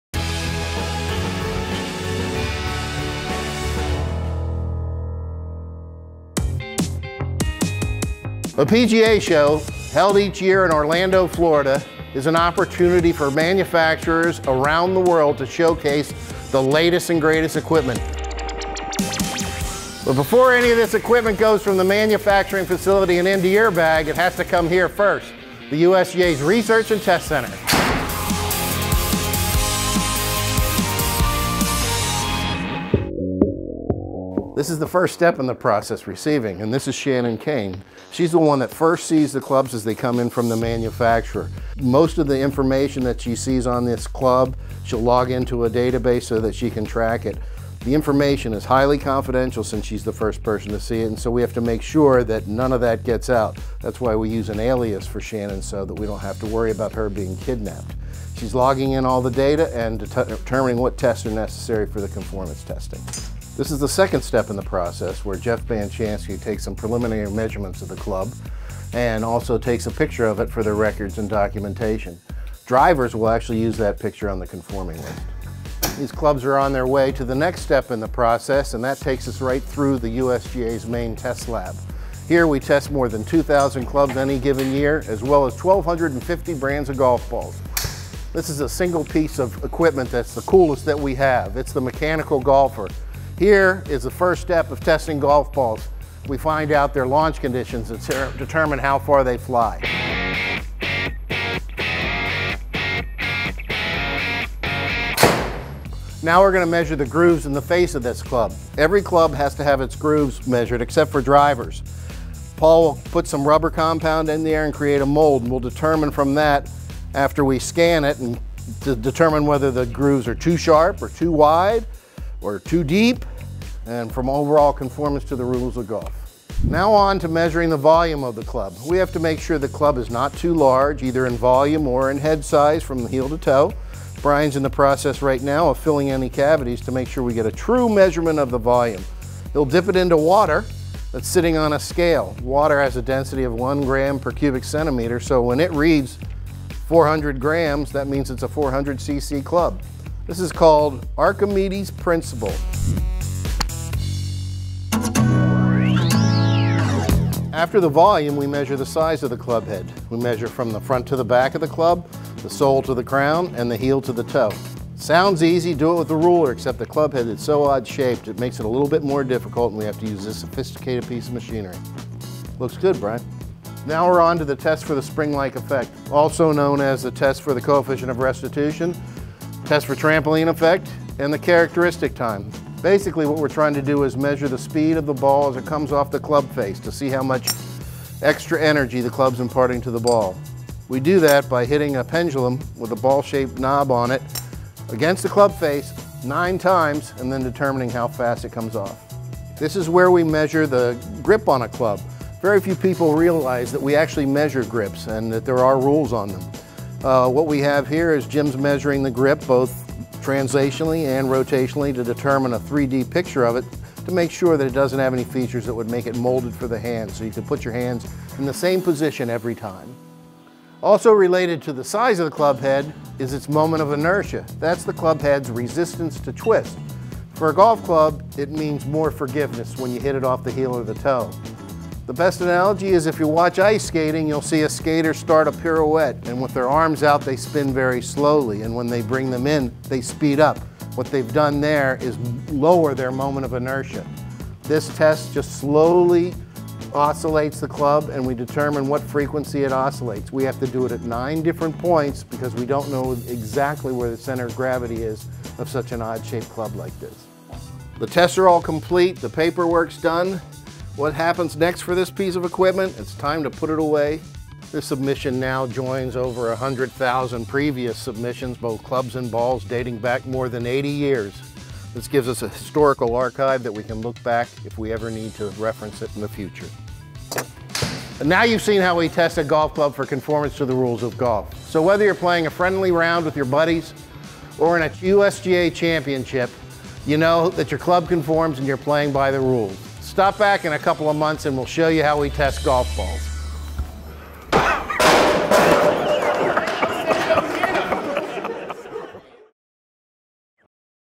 USGA Golf Journal: Inside the USGA Research and Test Center
In this Golf Journal, get an exclusive look inside the USGA Research and Test Center in Liberty Corner, N.J., where our engineers and experts put every piece of golf equipment through a rigorous testing process to ensure it conforms to the Rules of Golf.